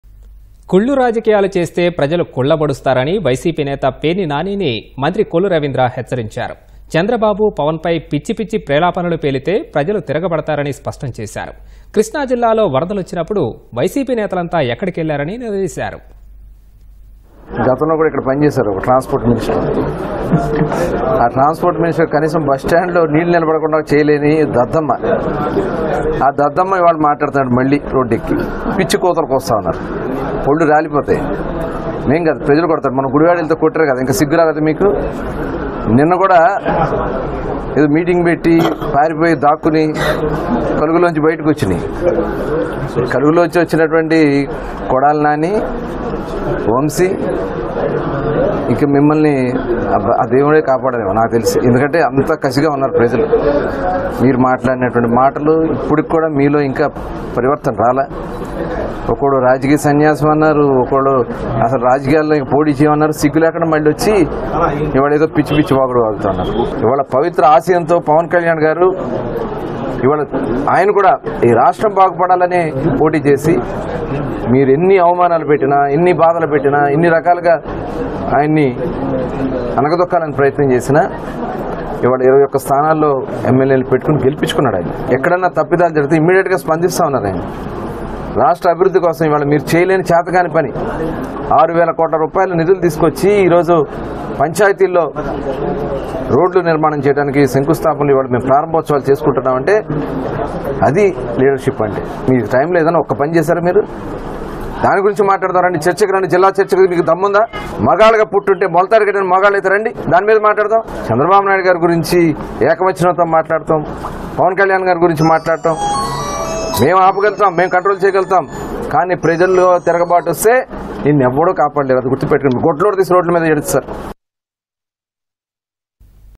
పేర్నినాని నోటికొచ్చినట్లు వాగితే పళ్లురాలిపోతాయ్ | Minister Kollu Ravindra Warning To Perni Nani
కుళ్లు రాజకీయాలు చేస్తే ప్రజలు కుళ్లబొడుస్తారని YCP నేత పేర్ని నానిని  మంత్రి కొల్లు రవీంద్ర హెచ్చరించారు. చంద్రబాబు, పవన్ పై పిచ్చిపిచ్చి ప్రేలాపనలు పేలితే  ప్రజలు తిరగబడతారని..స్పష్టం చేశారు. కృష్ణా జిల్లాలో వరదలు వచ్చినప్పుడు YCP నేతలంతా ఎక్కడికెళ్లారని నిలదీశారు.